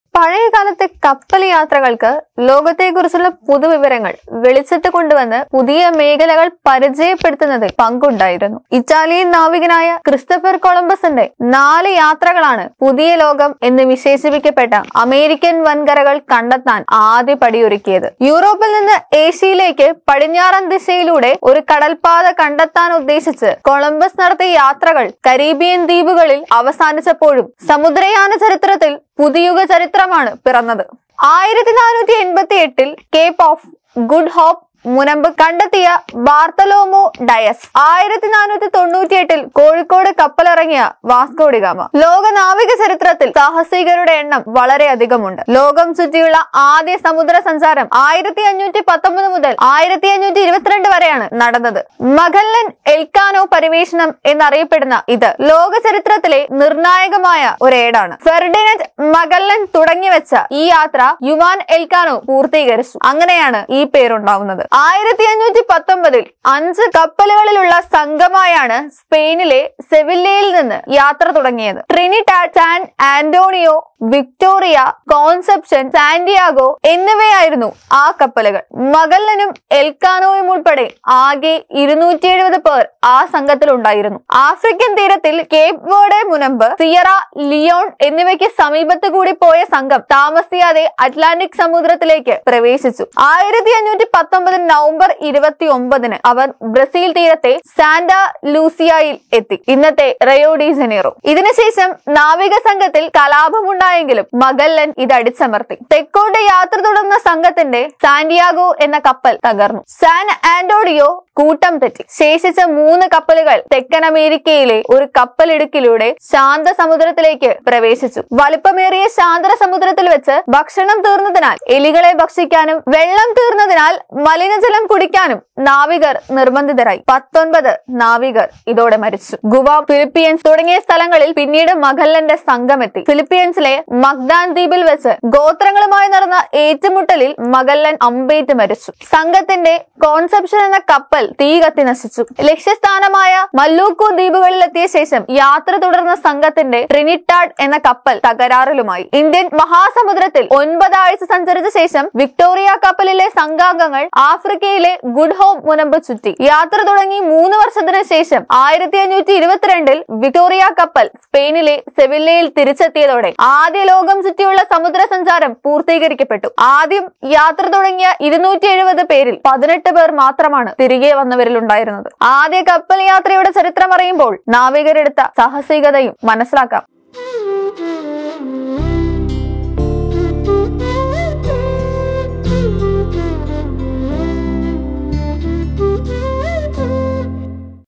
നാവികരുടെ കഠിന യാത്ര | The Discovery Of America | Ship Sail History
Sailing in the past played a role in bringing to light new information about the world and introducing new areas. The 4 voyages of the Italian navigator Christopher Columbus were the first steps to discover the American mainland, which was described as the New World. | പഴയകാലത്തെ കപ്പൽ യാത്രകൾക്ക്  ലോകത്തെക്കുറിച്ചുള്ള പുതുവിവരങ്ങൾ വെളിച്ചത്തുകൊണ്ടുവന്ന് പുതിയ മേഖലകൾ പരിചയപ്പെടുത്തുന്നതിൽ പങ്ക് ഉണ്ടായിരുന്നു. ഇറ്റാലിയൻ നാവികനായ ക്രിസ്റ്റഫർ കൊളംബസിന്റെ 4 യാത്രകളാണ്  പുതിയ ലോകം എന്നു വിശേഷിപ്പിക്കപ്പെട്ട അമേരിക്കൻ വൻകരകൾ കണ്ടെത്താൻ ആദ്യപടിയൊരുക്കിയത്.

"നാടിനൊപ്പം നേരിനൊപ്പം"